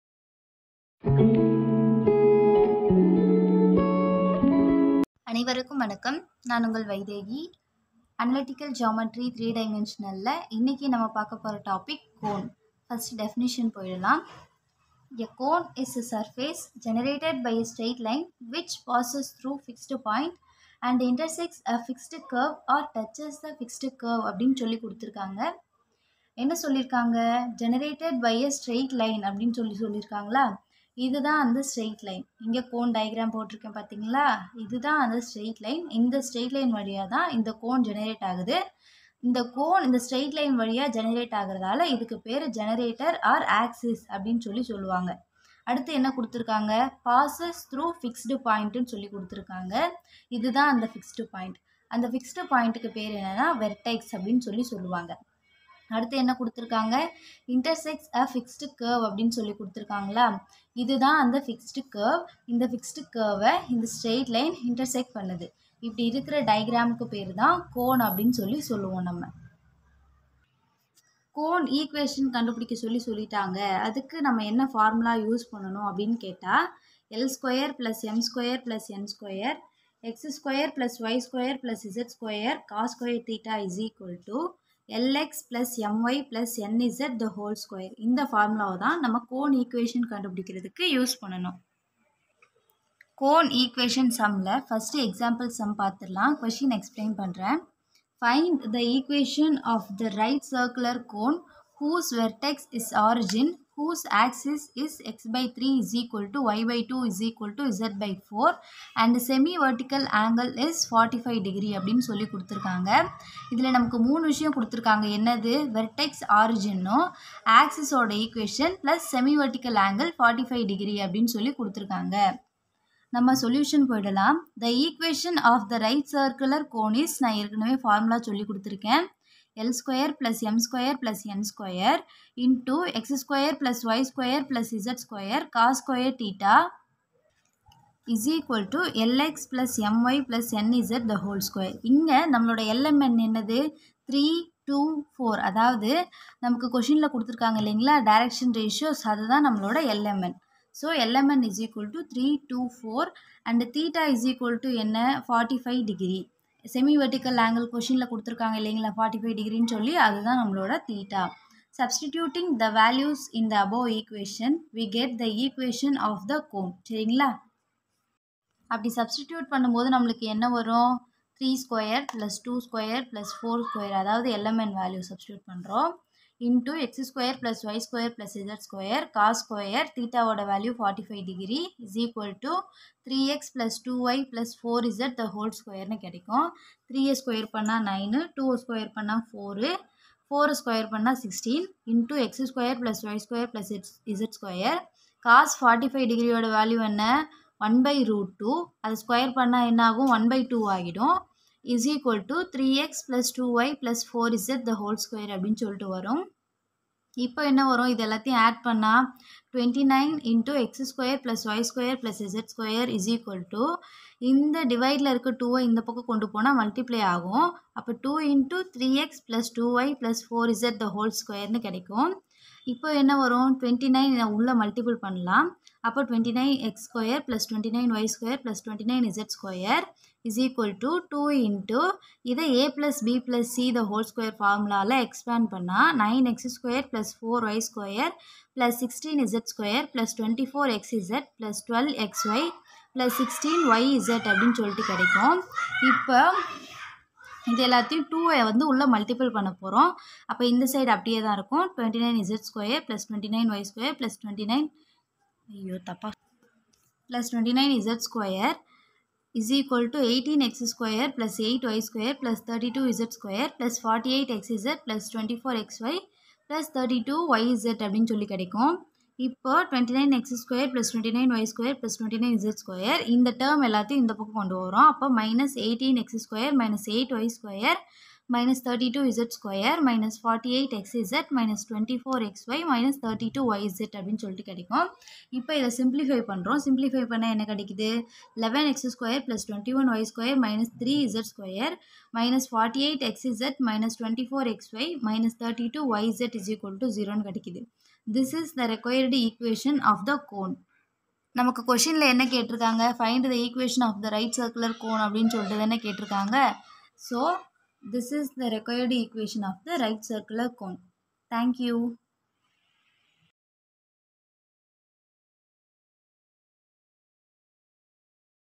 Analytical Geometry -3D (cone) in Tamil | THIRUVALLUVAR UNIVERSITY
Find the equation of the right circular cone whose vertex is origin ,who axis is x/3=y/2=z/4 and semi vertical angle is 45